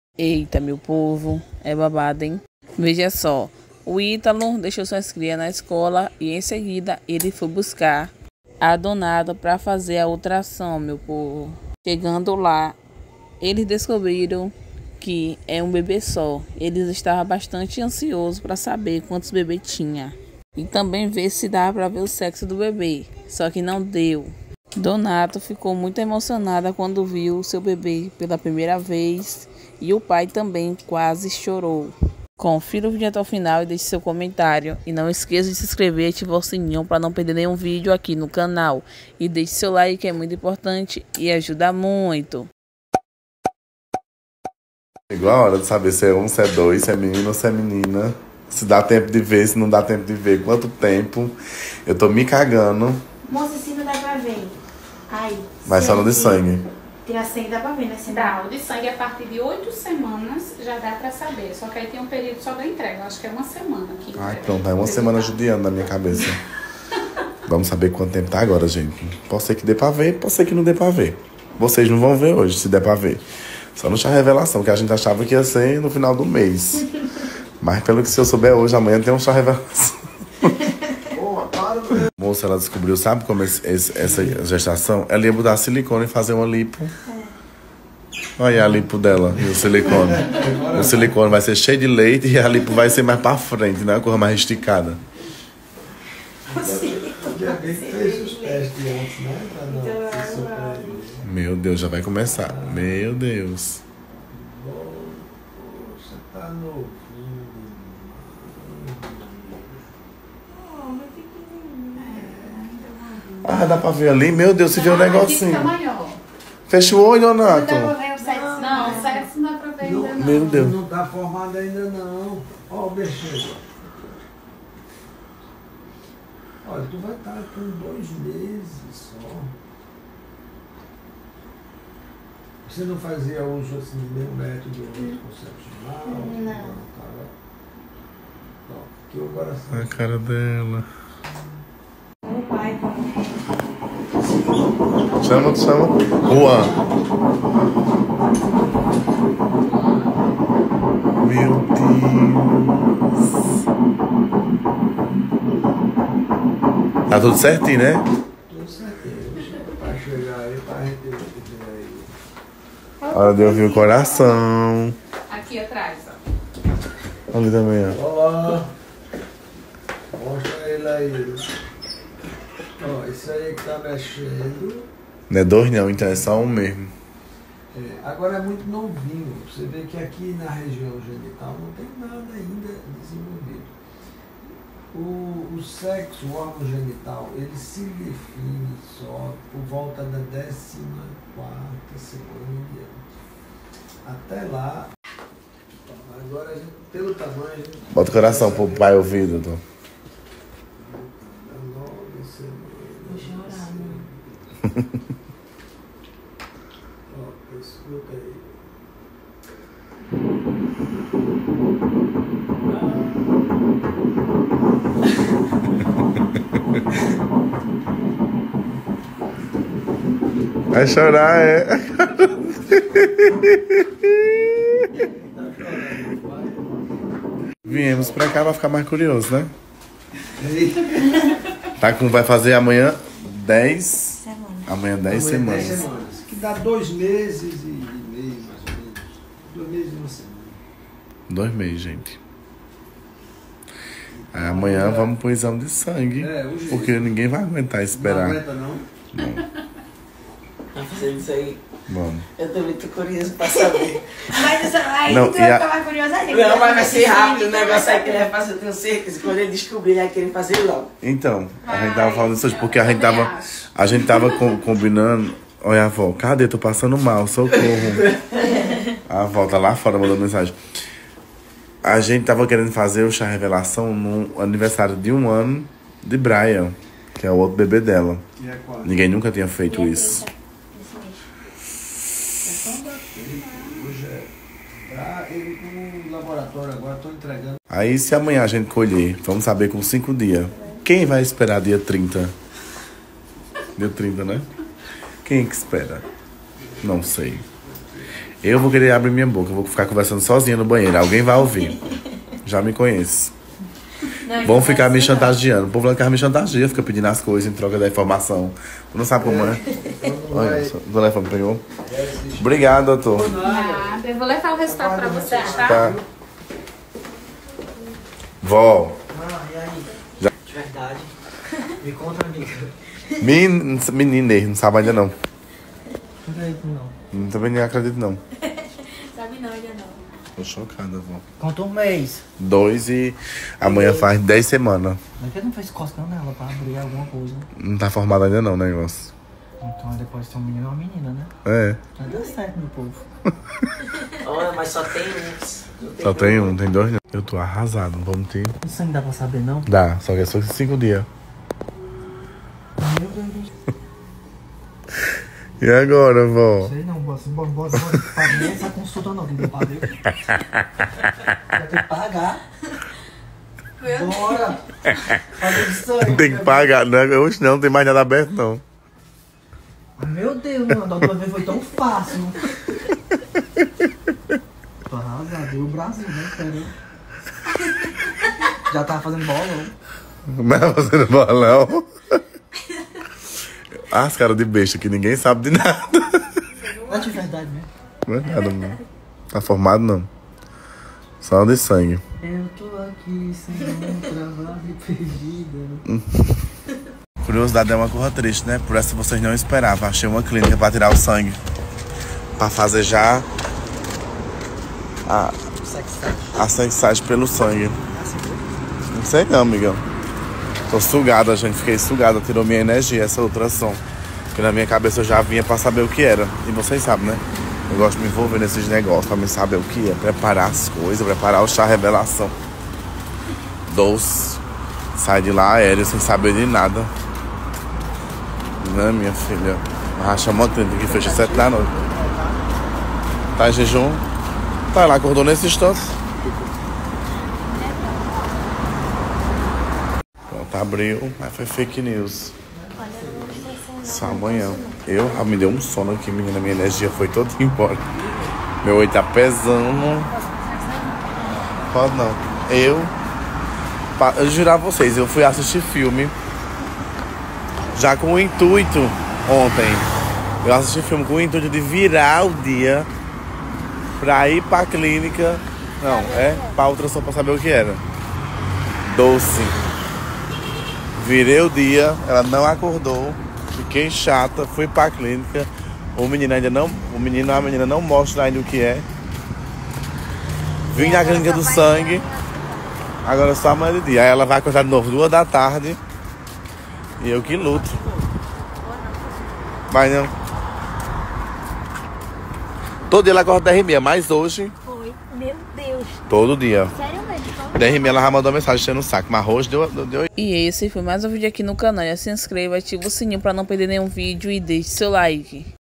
HYTALO LEVA DONATO PRA VER SE E DOIS BEBÊS😱REAÇÃO DELES😍
Hytalo Santos se diverte com a sua turma. Fiquem por dentro de tudo aqui no canal.